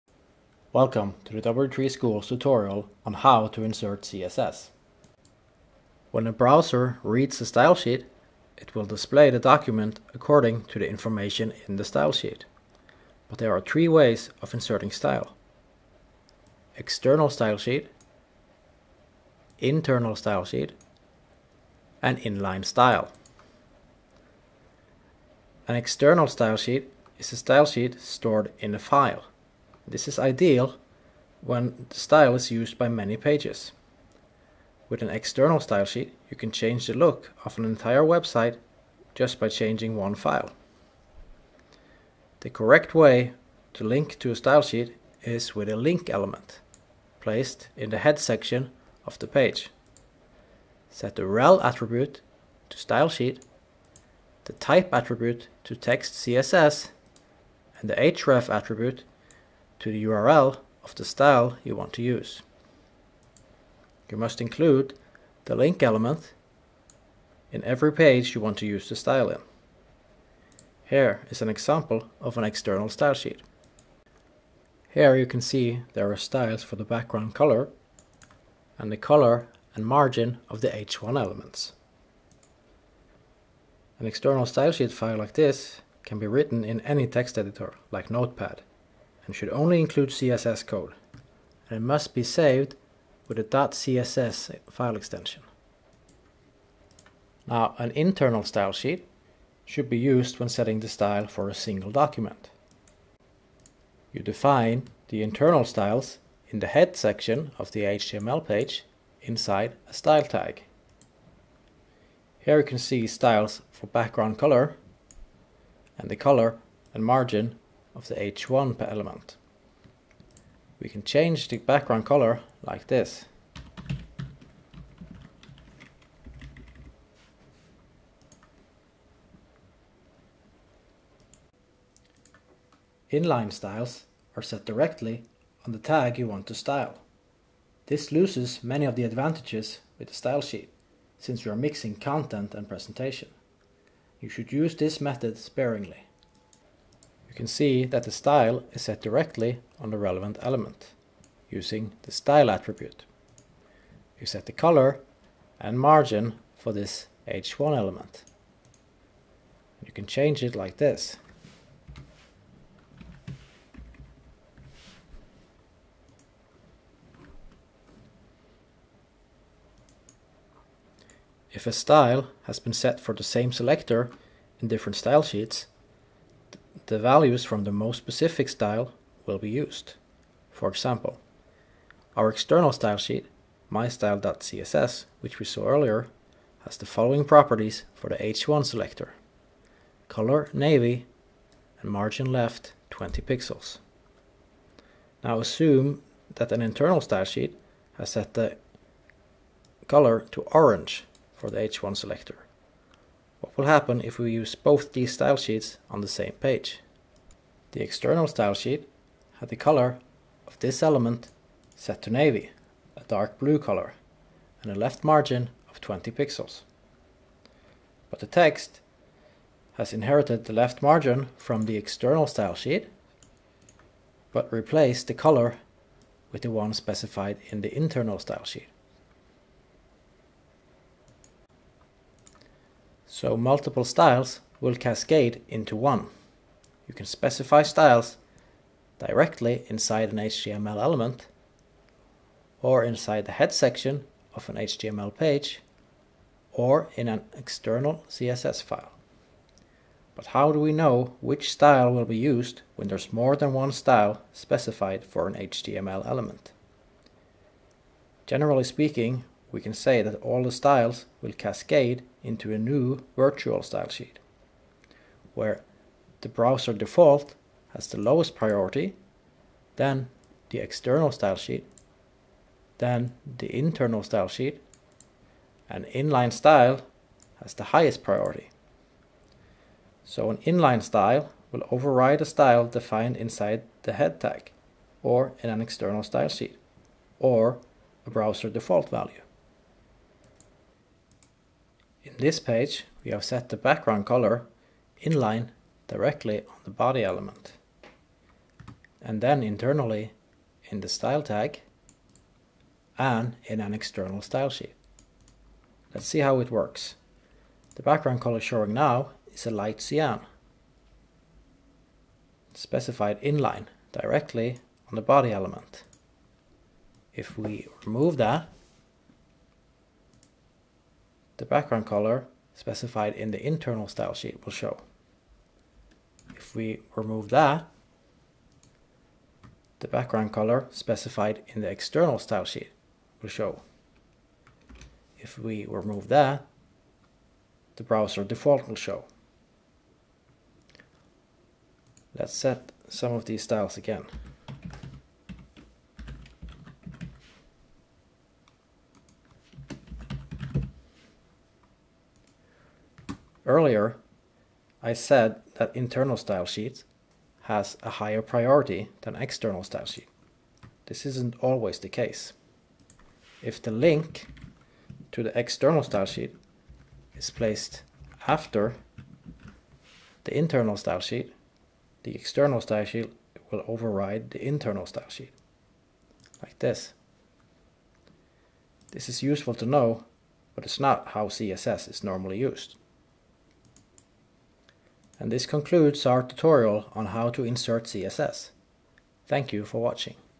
W3Schools How To Insert CSS Tutorial
Video tutorial from the CSS How To chapter of the CSS tutorial on w3schools.com